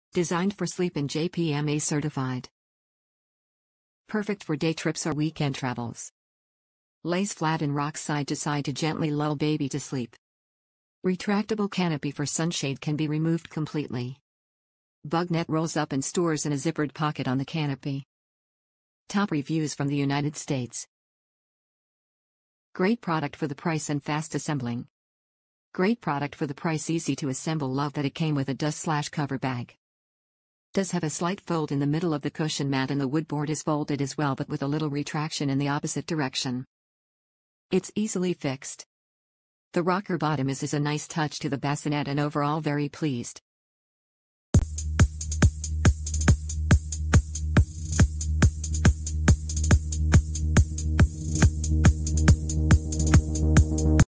Safety 1ˢᵗ® Nap & Go Rocking Bassinet with Bug Net, Trinity
Designed for sleep and JPMA certified
Perfect for day trips or weekend travels
Lays flat and rocks side to side to gently lull baby to sleep
Retractable canopy for sunshade can be removed completely
Bug net rolls up and stores in a zippered pocket on the canopy

Great product for the price and fast assembling
Great product for the price easy to assemble love that it came with a dust/cover bag. Does have a slight fold in the middle of the cushion mat and the wood board is folded as well but with a little retraction in the opposite direction. It’s easily fixed.
The rocker bottom is is a nice touch to the bassinet and over all very pleased!